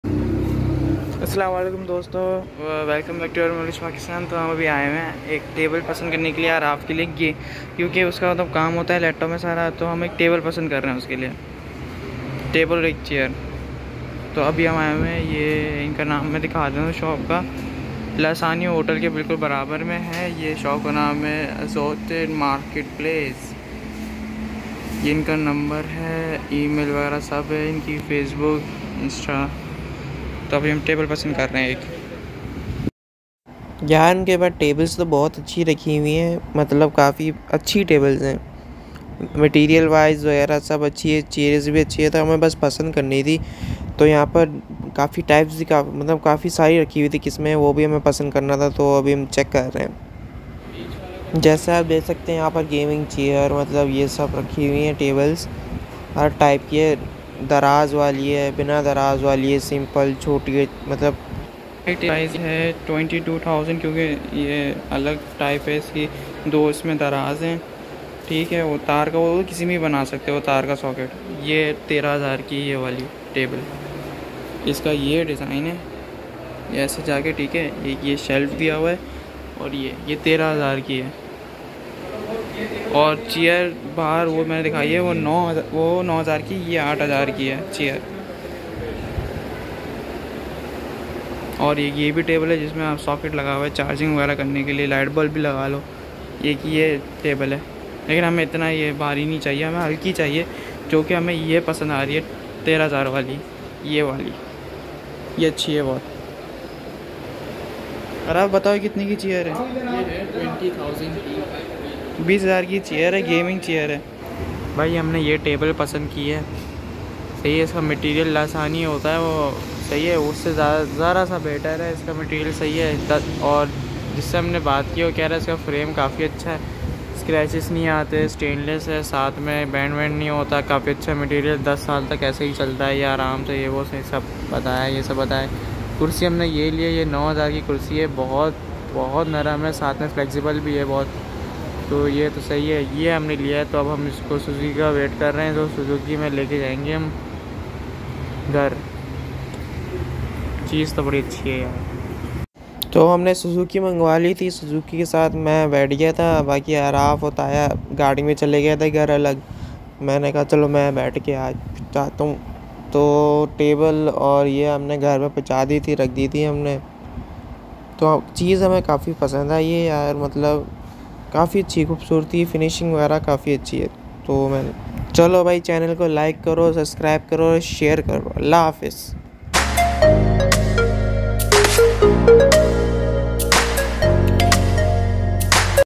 Best material tables and chairs | aoo or lekar jao 😃😆 Assorted Marketplace block 10
today, we bought a table and chair from the Assorted Marketplace best material tables and chairs under reasonable prices.

location is available in Google 👇
just type Assorted Marketplace